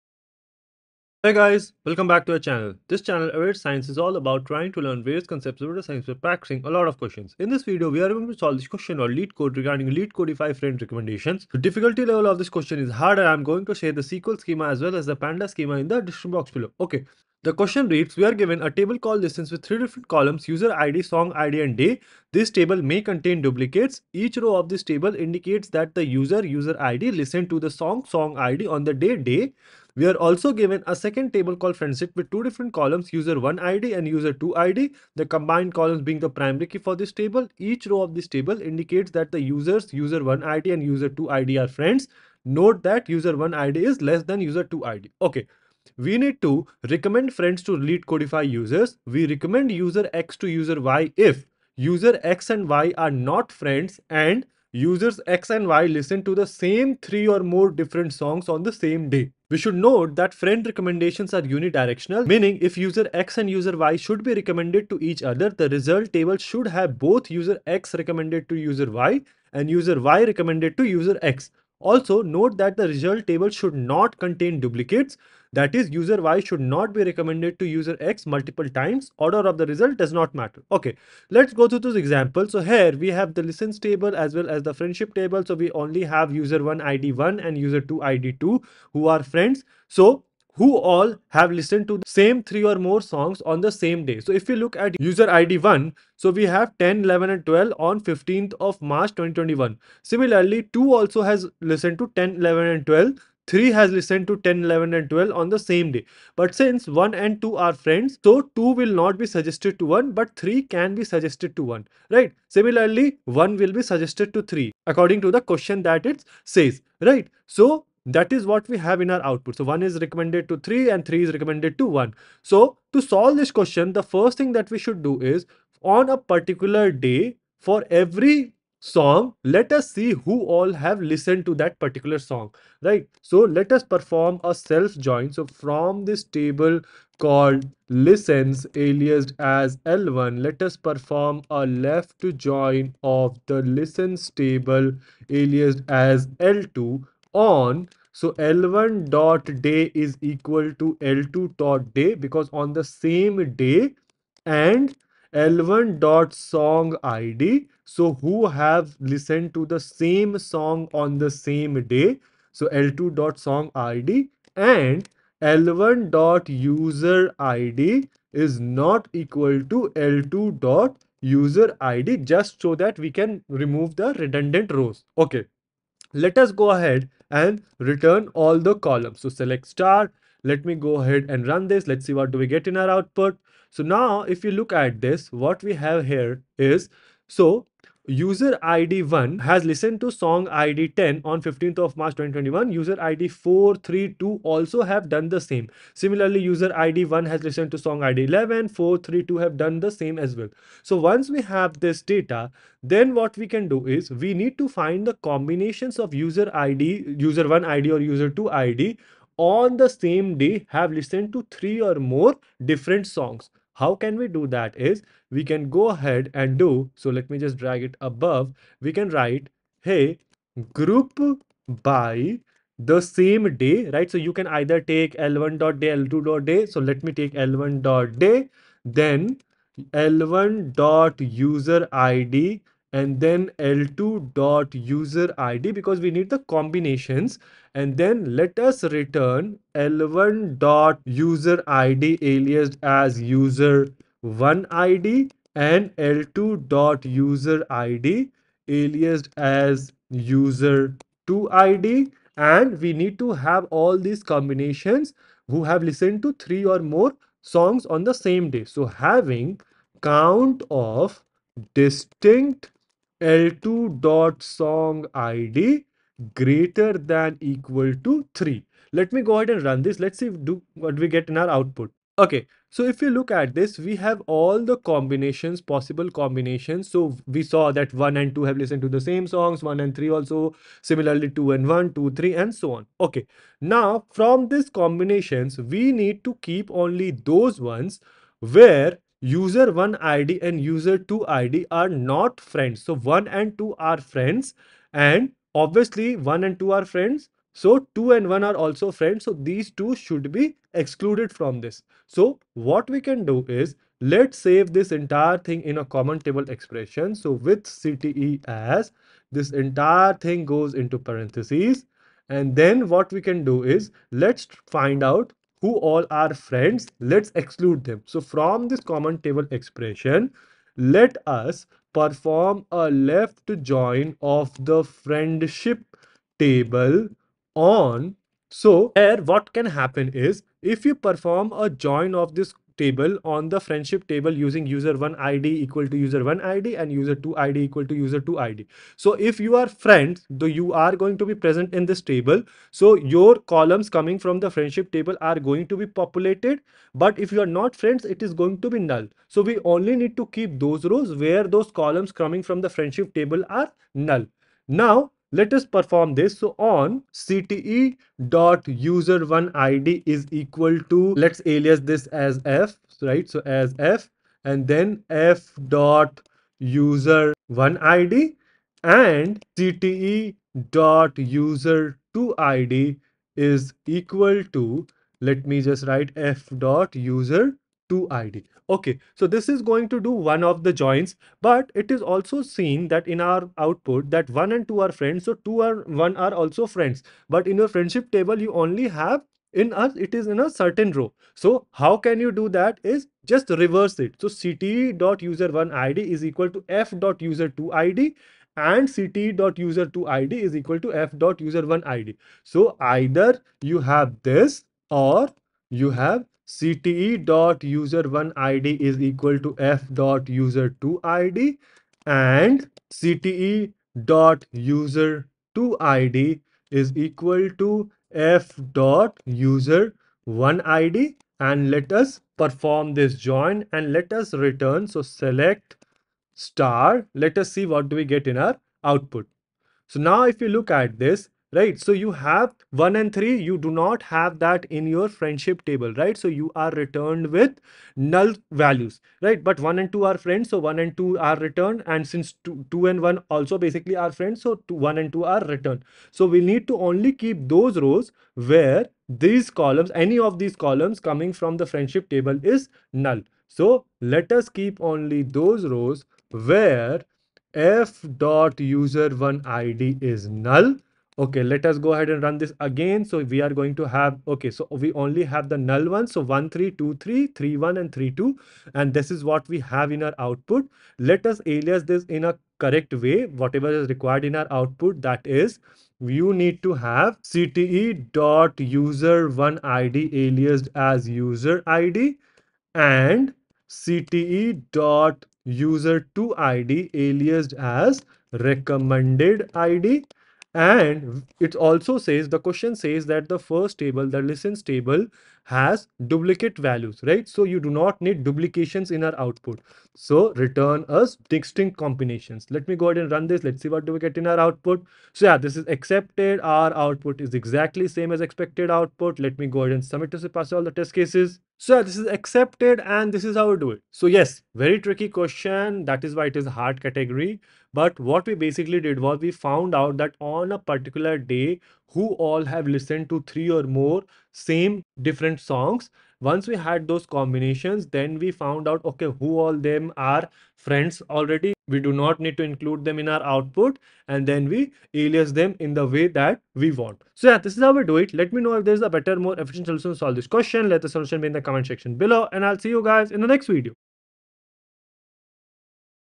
Leetcode HARD 1917 - Leetcodify Friends Recommendations: Explained by Everyday Data Science
SQL Schema:
Create table If Not Exists Listens (user_id int, song_id int, day date)
Create table If Not Exists Friendship (user1_id int, user2_id int)
Truncate table Listens
insert into Listens (user_id, song_id, day) values ('1', '10', '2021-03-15')
insert into Listens (user_id, song_id, day) values ('1', '11', '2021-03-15')
insert into Listens (user_id, song_id, day) values ('1', '12', '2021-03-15')
insert into Listens (user_id, song_id, day) values ('2', '10', '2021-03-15')
insert into Listens (user_id, song_id, day) values ('2', '11', '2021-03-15')
insert into Listens (user_id, song_id, day) values ('2', '12', '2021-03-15')
insert into Listens (user_id, song_id, day) values ('3', '10', '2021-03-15')
insert into Listens (user_id, song_id, day) values ('3', '11', '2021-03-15')
insert into Listens (user_id, song_id, day) values ('3', '12', '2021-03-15')
insert into Listens (user_id, song_id, day) values ('4', '10', '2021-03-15')
insert into Listens (user_id, song_id, day) values ('4', '11', '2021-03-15')
insert into Listens (user_id, song_id, day) values ('4', '13', '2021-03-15')
insert into Listens (user_id, song_id, day) values ('5', '10', '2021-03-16')
insert into Listens (user_id, song_id, day) values ('5', '11', '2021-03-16')
insert into Listens (user_id, song_id, day) values ('5', '12', '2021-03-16')
Truncate table Friendship
insert into Friendship (user1_id, user2_id) values ('1', '2')

Pandas Schema:
listens = pd.DataFrame(data, columns=['user_id', 'song_id', 'day']).astype({'user_id':'Int64', 'song_id':'Int64', 'day':'datetime64[ns]'})
data = [[1, 2]]
friendship = pd.DataFrame(data, columns=['user1_id', 'user2_id']).astype({'user1_id':'Int64', 'user2_id':'Int64'})